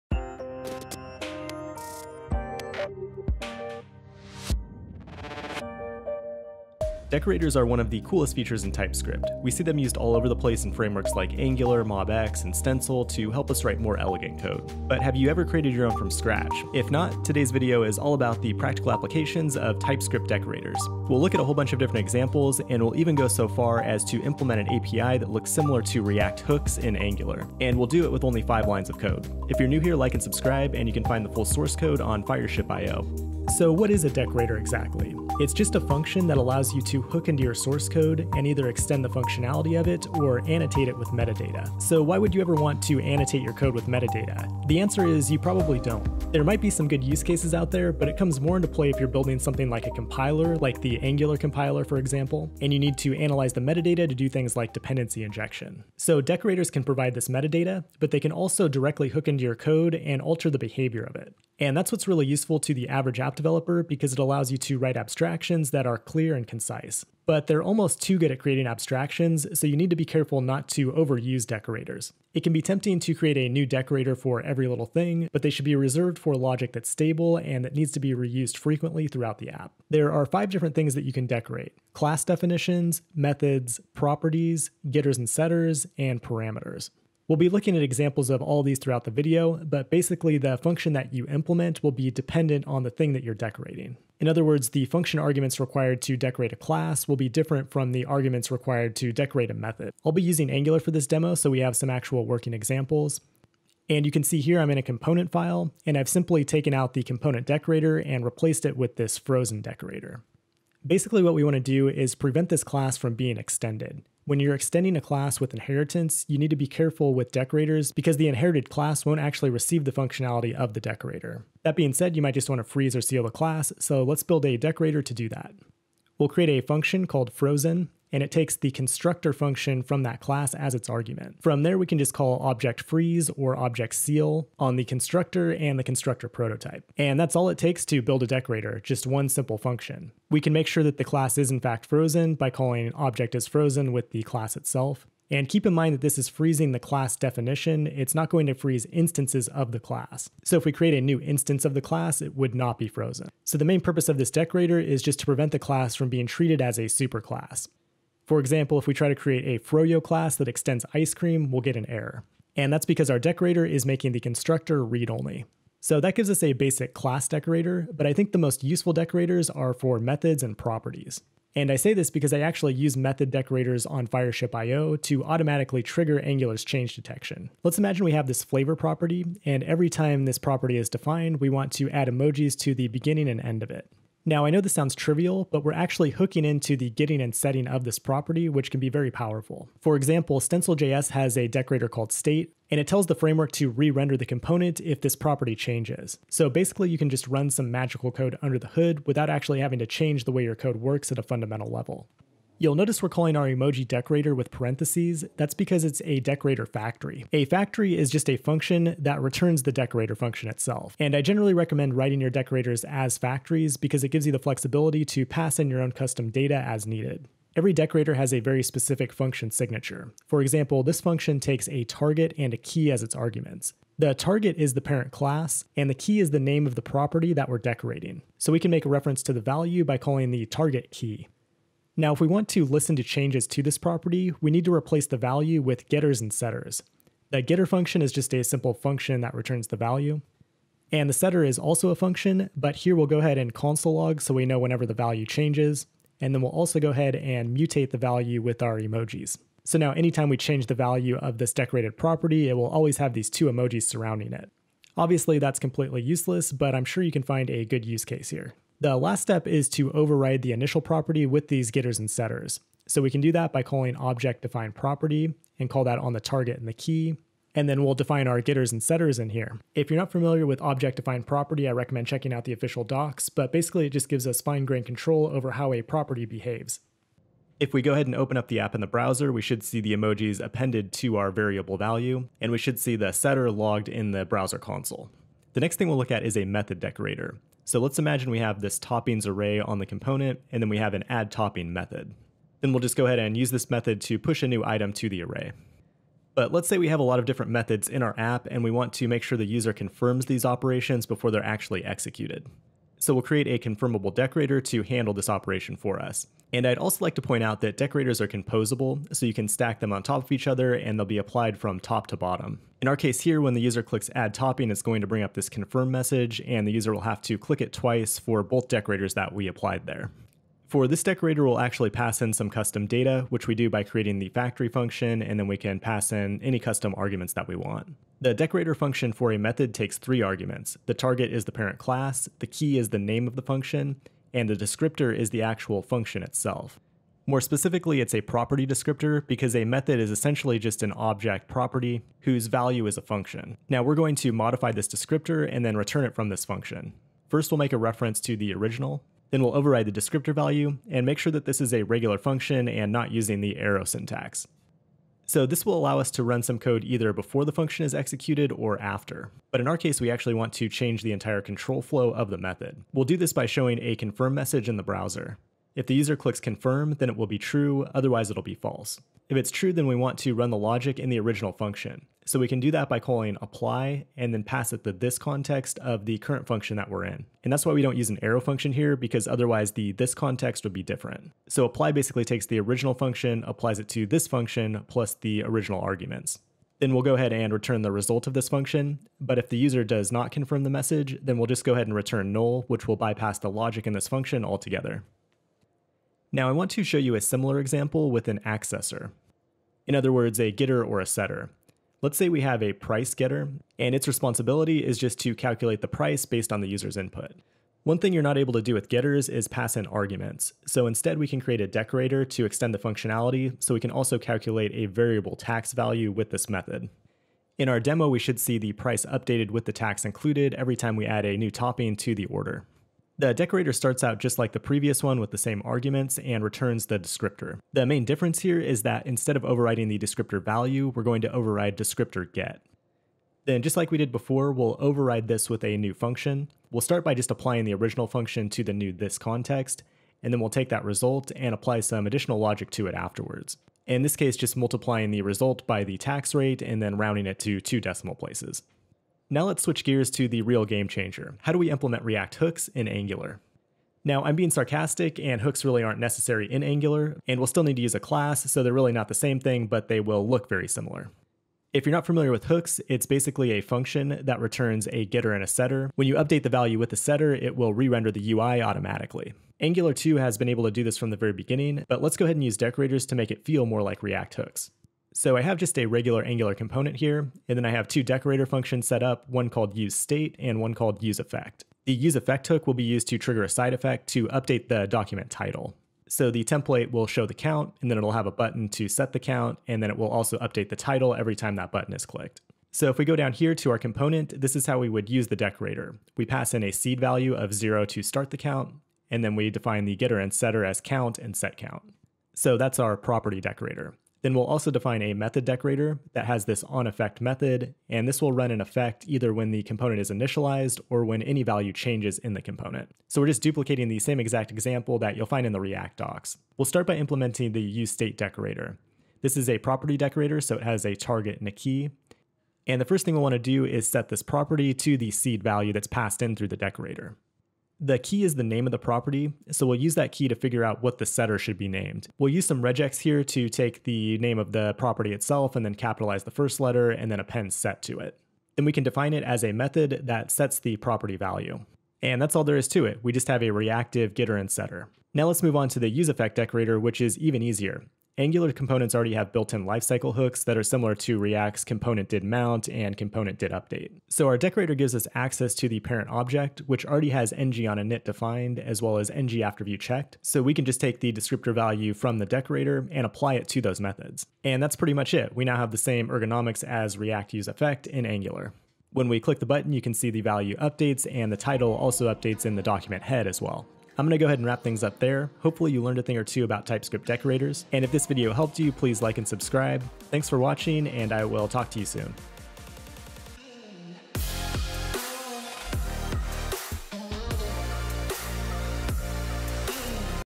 The Magic of TypeScript Decorators
Practical examples of TypeScript Decorators 🍨. Learn how to use decorators to build an API similar to React Hooks in Angular